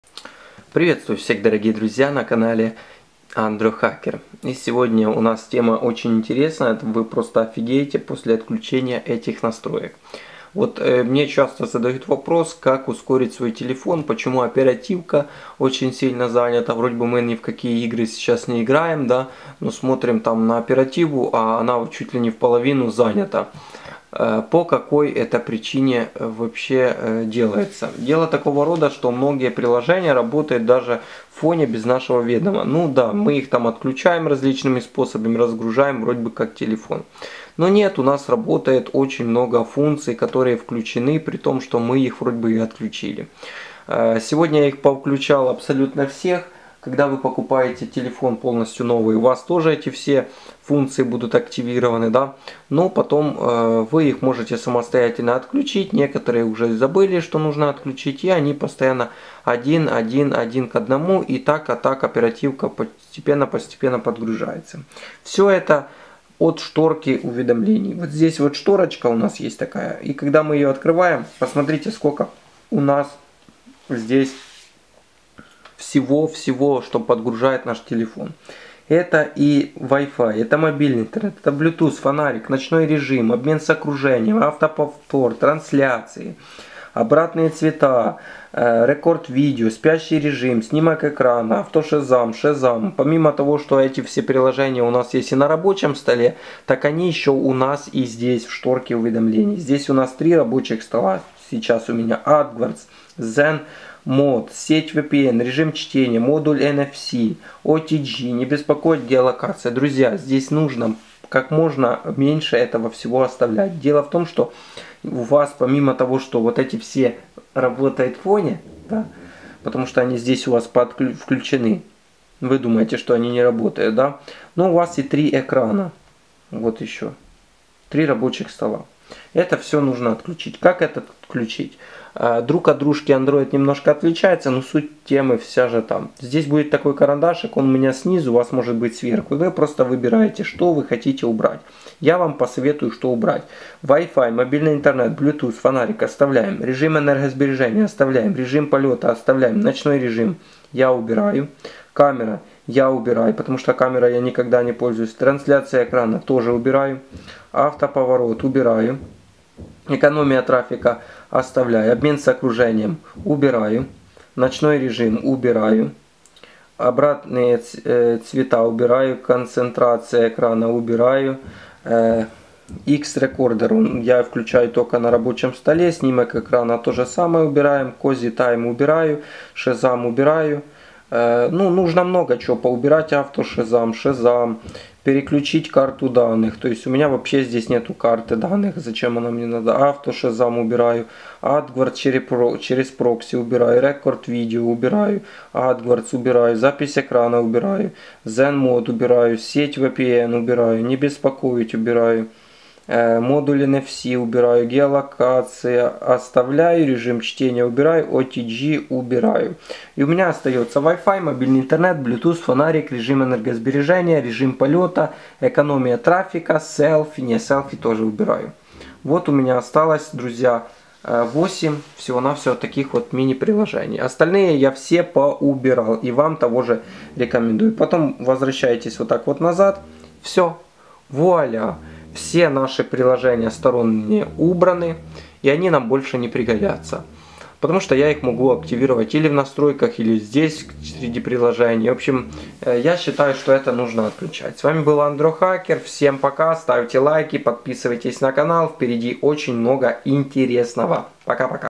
Ты ОФИГЕЕШЬ После Отключения ЭТОЙ настройки | Реальный способ разгрузить оперативную память телефона
Хочу вам показать еще один способ на вашем мобильном телефоне, который реально вам поможет разгрузить оперативную память. Многие приложения даже без нашего с вами разрешения постояно работают в фоне, многие мы уже успели отключить. Но вы даже не догадываетесь друзья что еще много что работает. Сегодня я вам и покажу что еще можно отключить то!